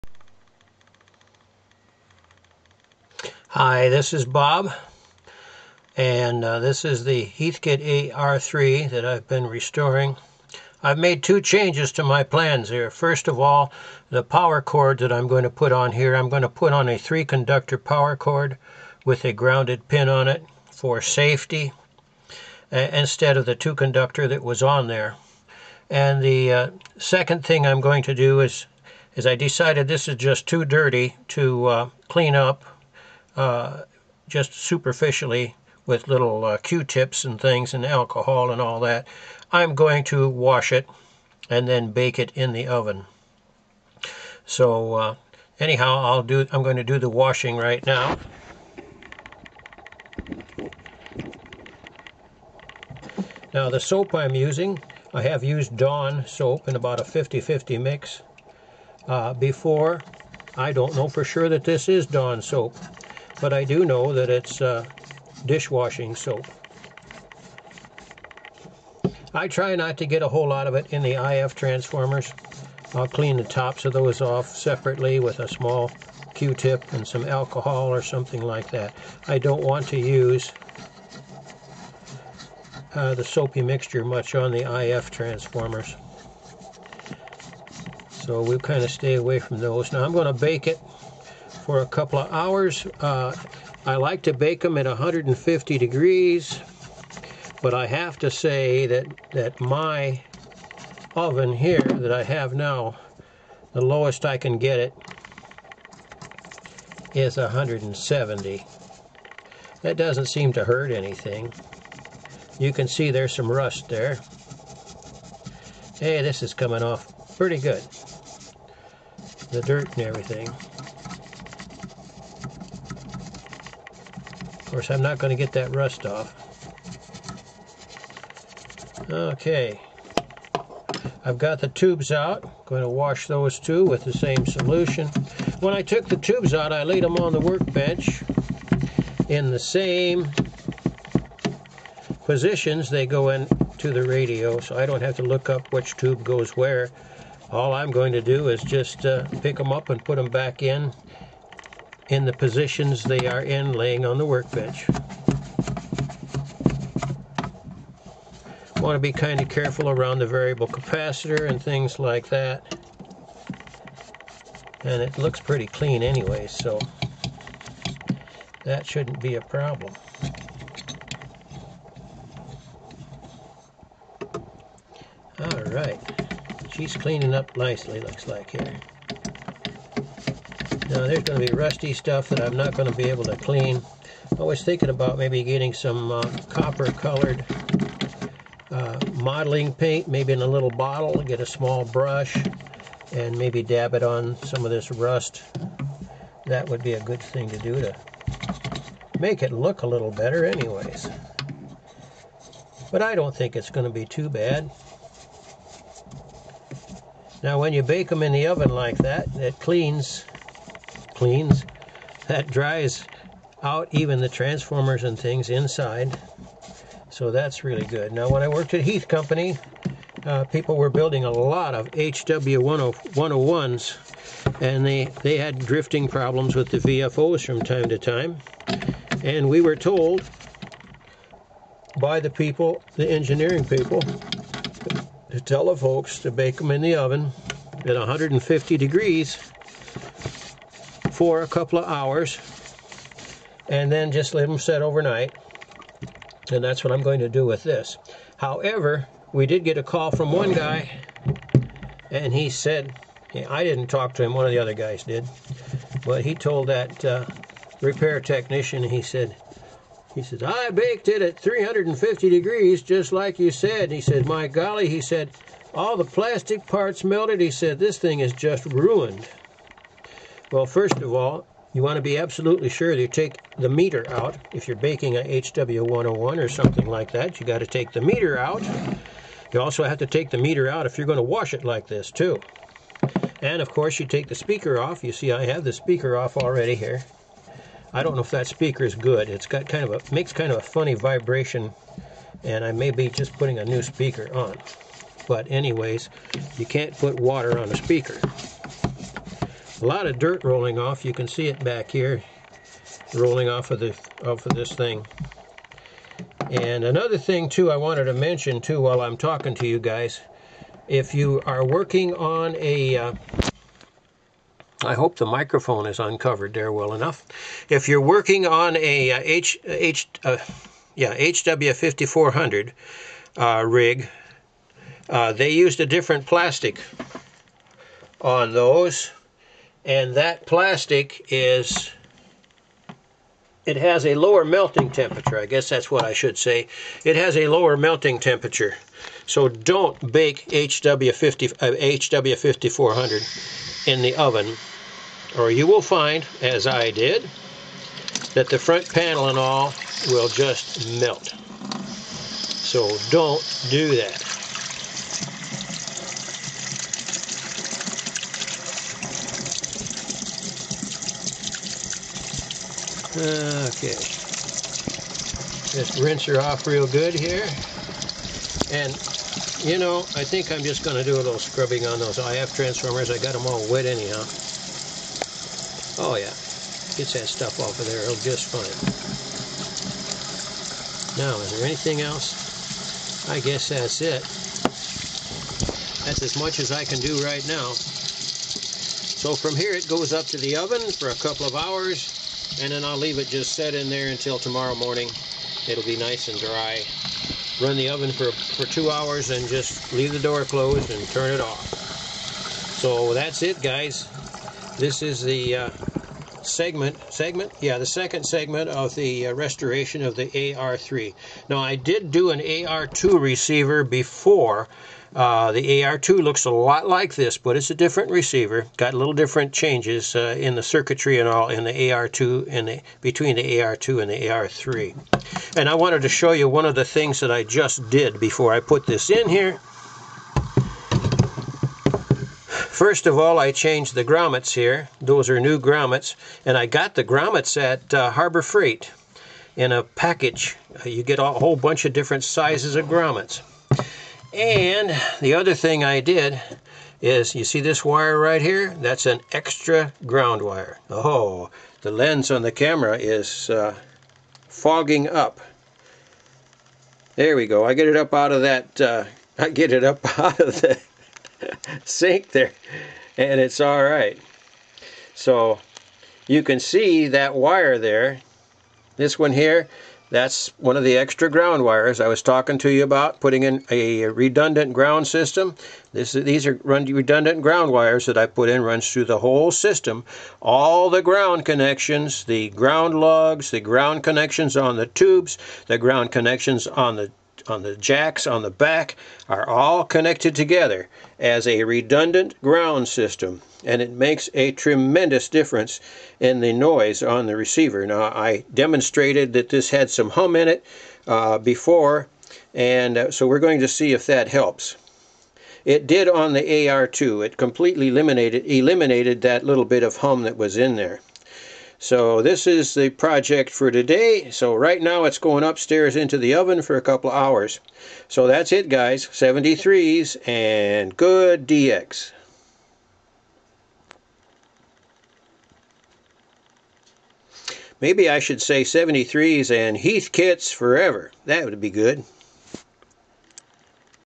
Part - 2 , HEATHKIT AR-3, Amateur Radio Hf receiver repair
Part-2 of the Heathkit AR-3 repairs . I call this a repair and not a restoration because there are many more things usually done in a restoration such as removing lots of parts and painting the chassis with copper color paint etc. AS I completed part-1  with the controls all lubricated and working freely the AR_3 was turned on  and there was  the local AM broadcast station coming in weakly . Knowing the AR-3 to be a very sensitive receiver it was late evening  I attached a 6 foot piece of hookup wire laying on the work bench to the antenna terminal and was able to tune in many AM  broadcast stations all over the country . I was surprised at this because I thought there would be a lot more repairs before  the AR-3 would be receiving anything . Heathkit did an amazing job with the AR-3  I really like it .  This video is about washing the AR-3 and installing a " redundant ground system"  to repair poor grounding in the chassis due to the unit sitting for so many years possibly in a basement or garage also installed today  were new grommets and dial light bulbs the bulbs used were type 1847 the AR-3 manual calls for type 47 the 1847 bulbs are the heavy duty 47 commercial bulbs and have a much longer life . There is that ticking or clicking in the background on this video just like the part -1 video since this is about 20 feet from the workbench where I made the first video I suspect that noise is in the camera I am using  I will try a different camera for the Part-3 video to see if this helps I do not think the noise is loud enough to be disruptive but I will try another camera to see if it is in the camera I used .
   73 Heathkits forever  Bob